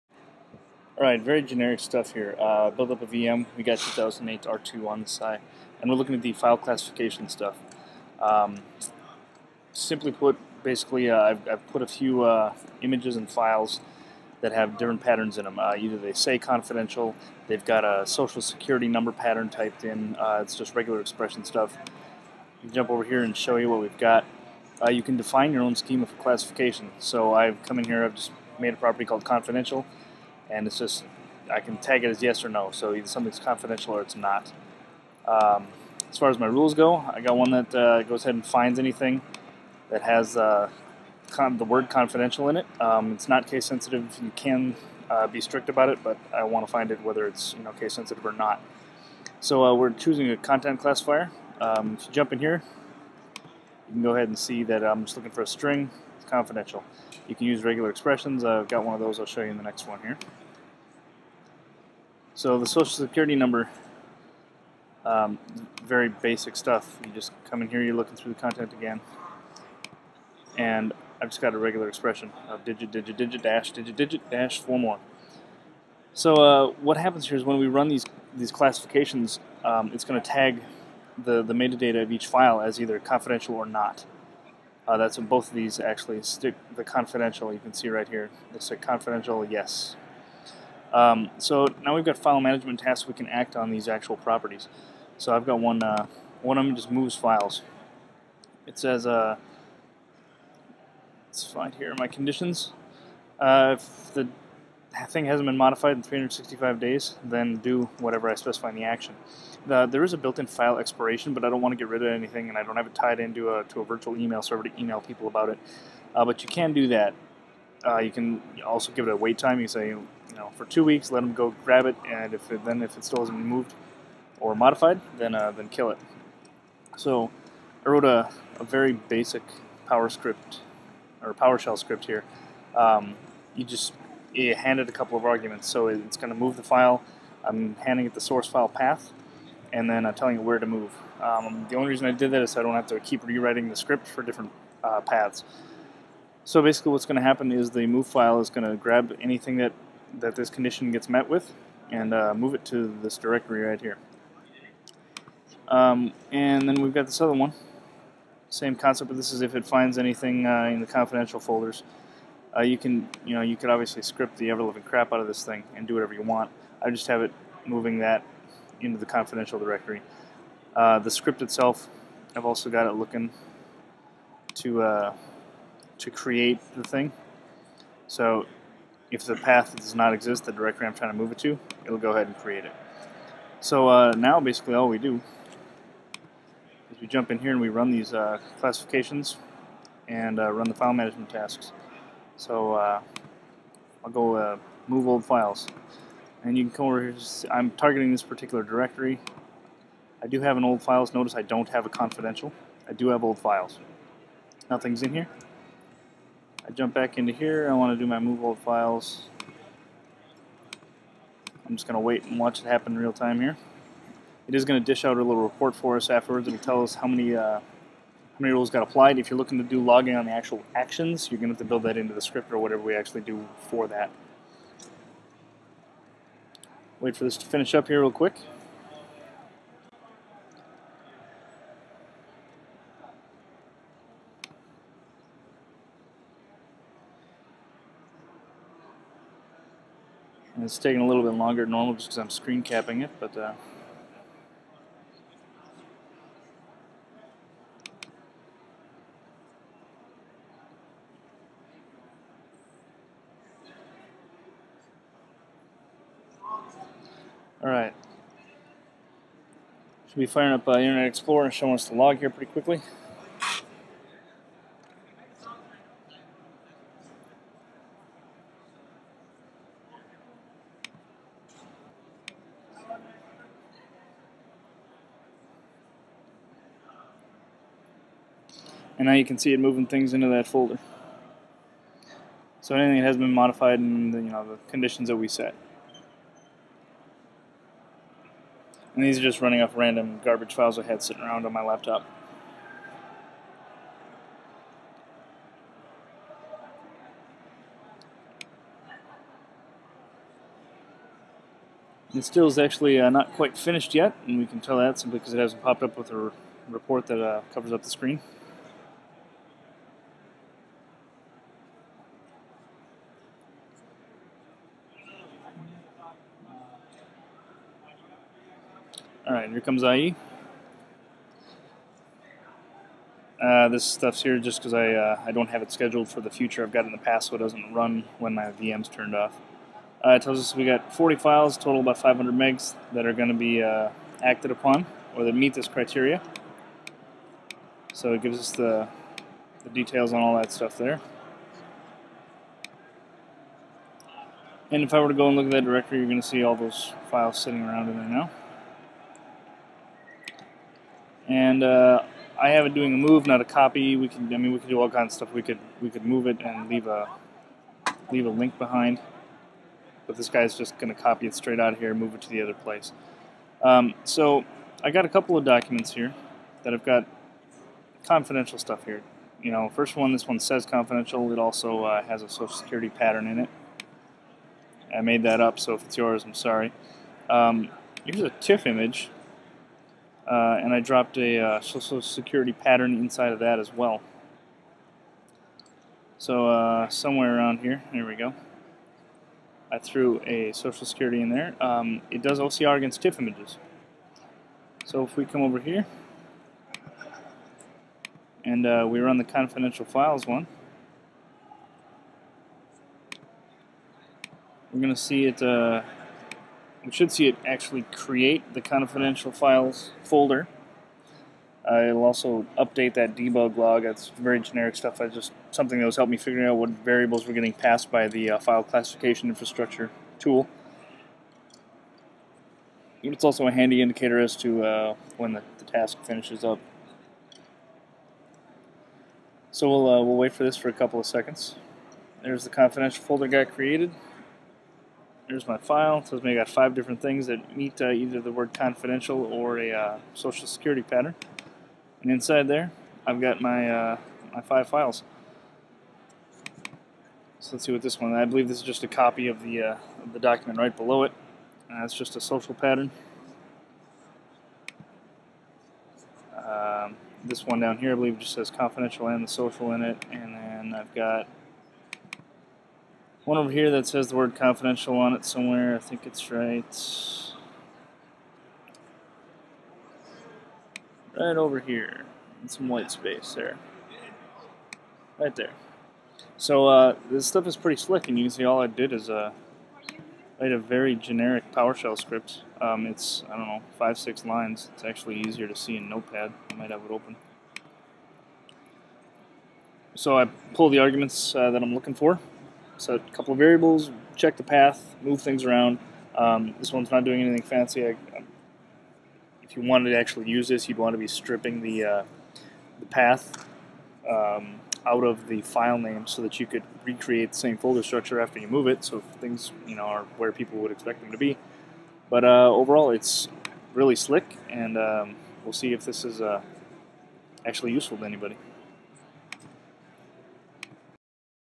FCI
Quick run through of rudimentary setup of Windows File Classification structure and what it can be used for.